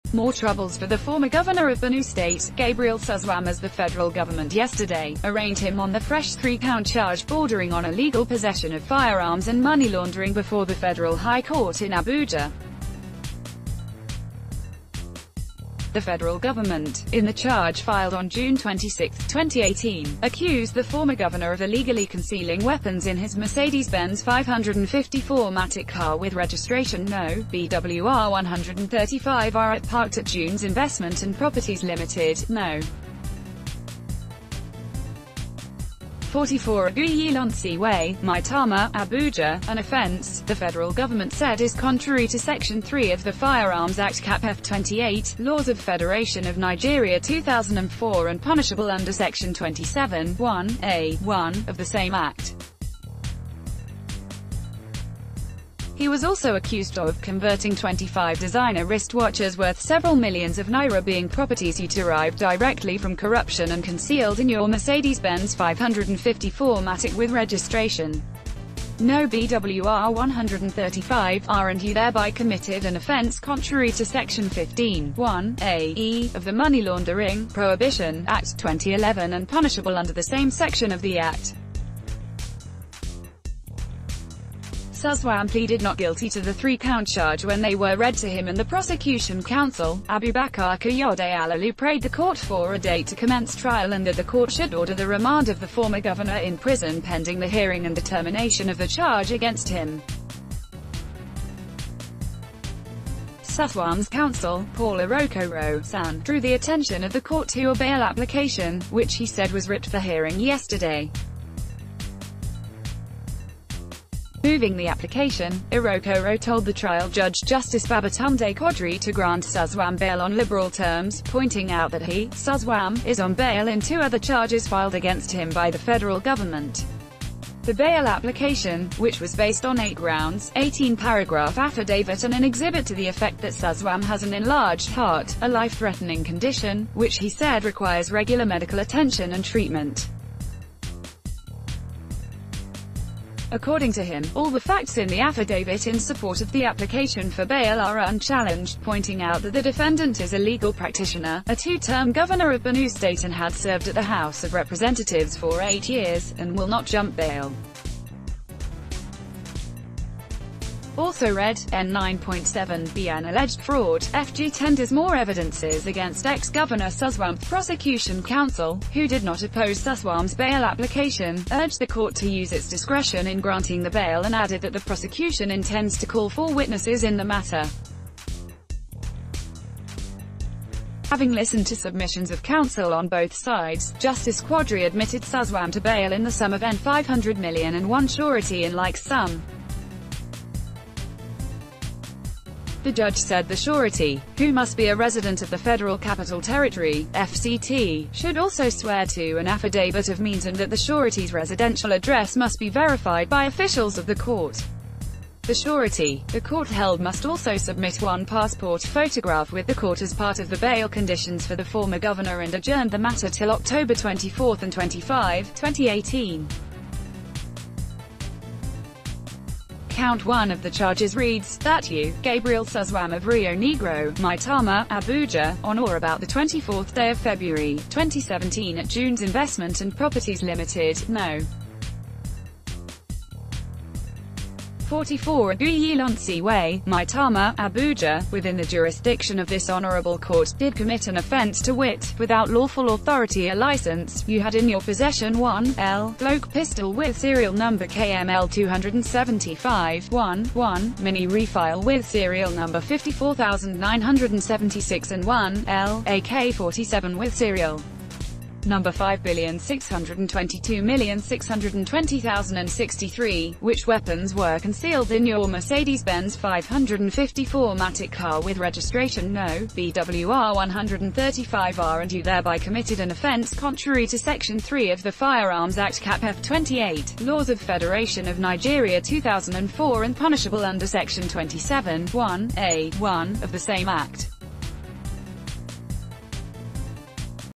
FG slams fresh three-count charge against Suswam
Charles K. Hundt